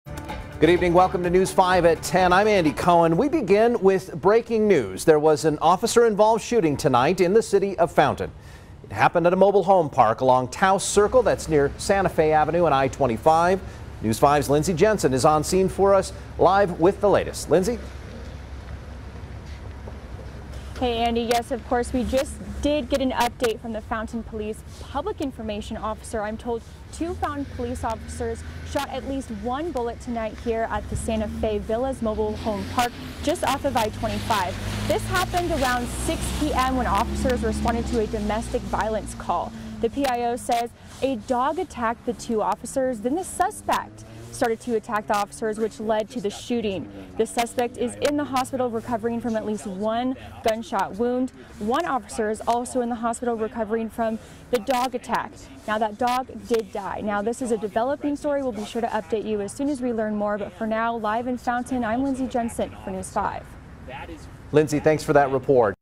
Fountain officers shoot suspect and dog after being attacked
Fountain police officers shot a man and his dog who attacked them when they responded to a domestic disturbance call Saturday night.